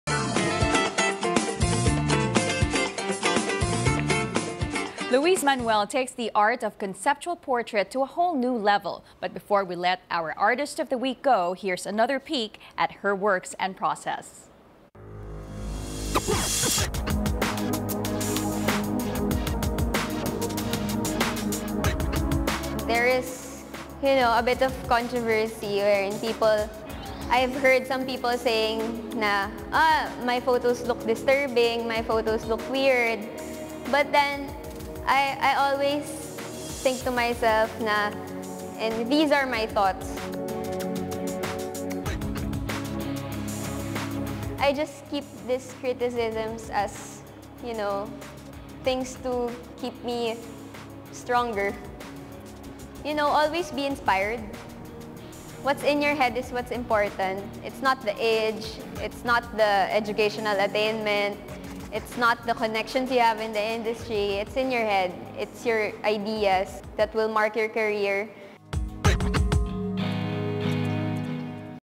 SOLAR DAYBREAK: ARTIST OF THE WEEK Louise Manuel
Louise Manuel takes the art of conceptual portrait to a whole new level.
Before we let our artist of the week go, here's another peek at her works and process.